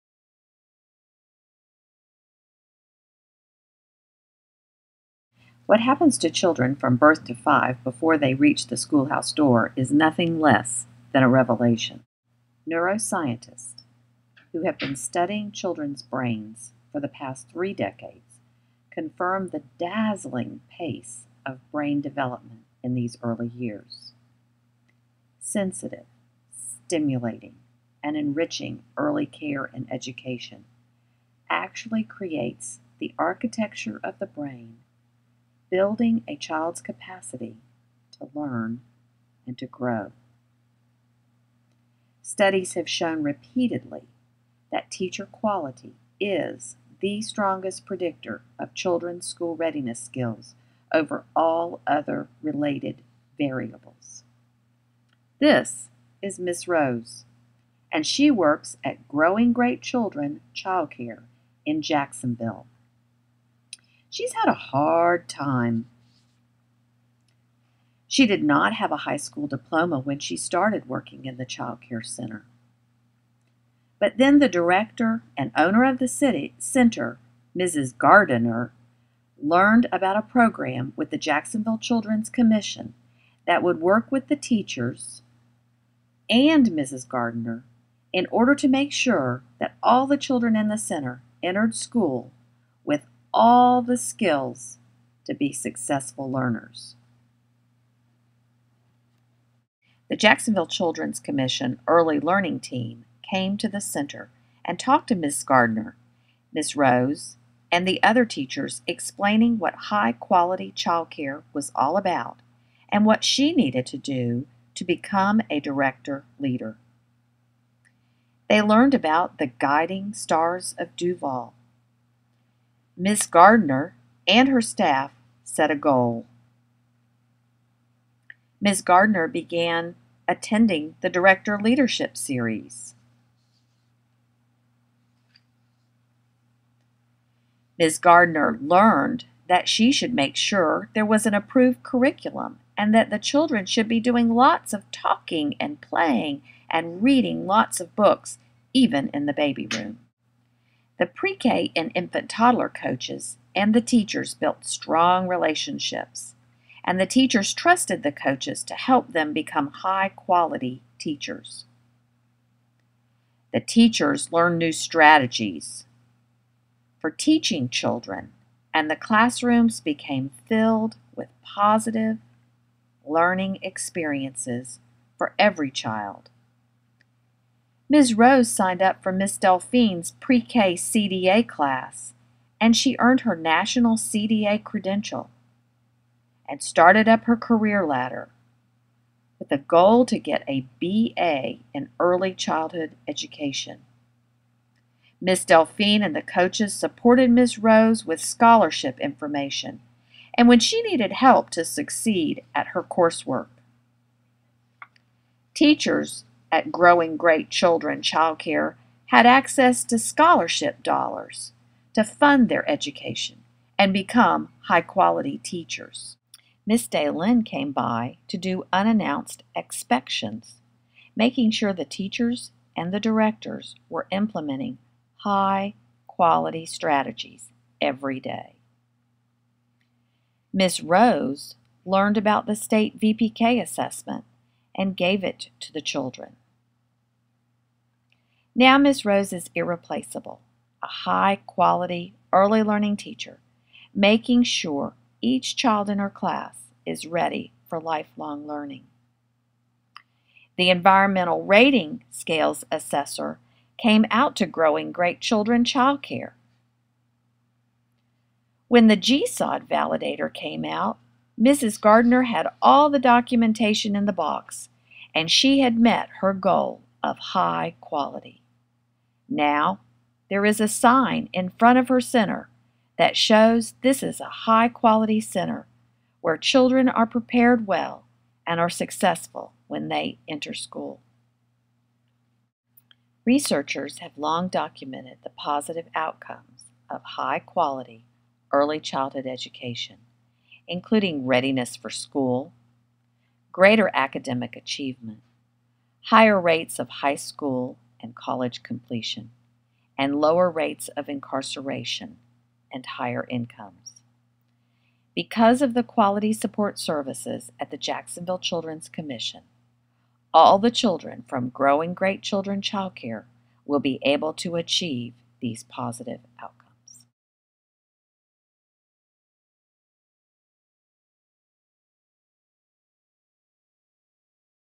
ELCM In Plain English.mov
Created for the Jacksonville Children's Commission, using the "In Plain English" format, to share with the Board of Directors the Early Learning Coaching Model.